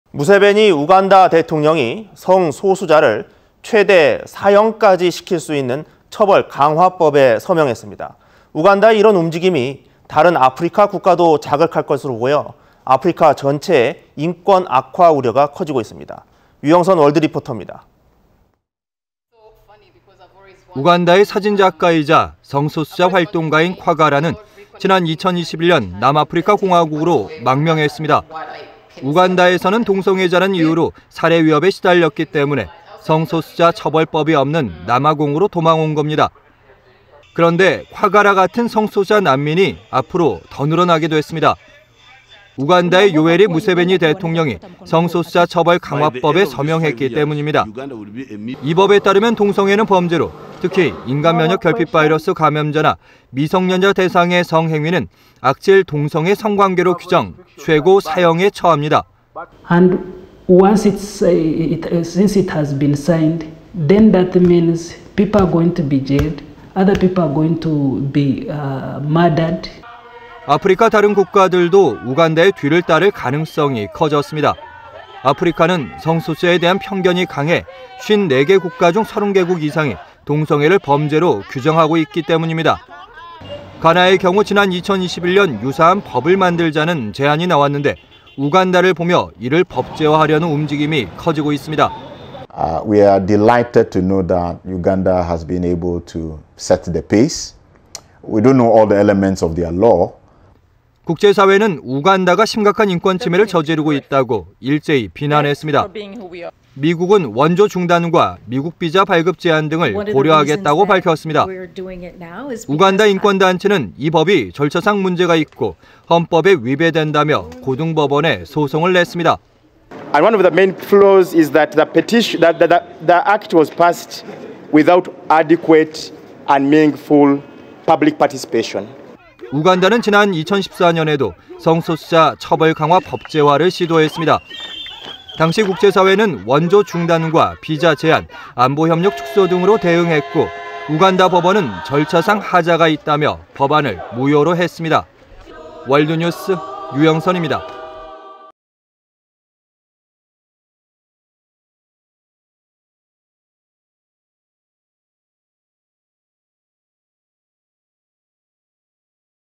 우간다 "동성애자 사형"…아프리카 번질까 / OBS 오늘의 월드뉴스
무세베니 우간다 대통령이
성소수자를 최대 사형까지
시킬 수 있는 처벌 강화법에
서명했습니다.
우간다의 이런 움직임이
다른 아프리카 국가도
자극할 것으로 보여
아프리카 전체의 인권 악화
우려가 커지고 있습니다.
유영선 월드리포터입니다.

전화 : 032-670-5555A